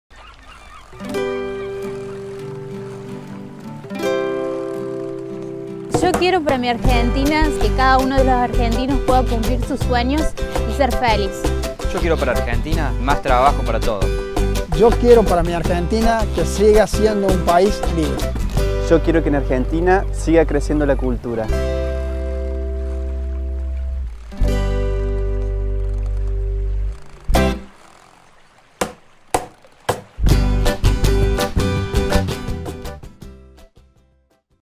Día de la Independencia 2014 - Spot 1 - UNVM
"Pensar en nuestra independencia como nación es un momento ideal para reflexionar sobre nuestra libertad... Esa que nos permite decidir y actuar pensando en la sociedad que queremos construir... PORQUE SOMOS LIBRES PARA HACER EL PAÍS QUE QUEREMOS"

9 de julio DIA DE LA INDEPENDENCIA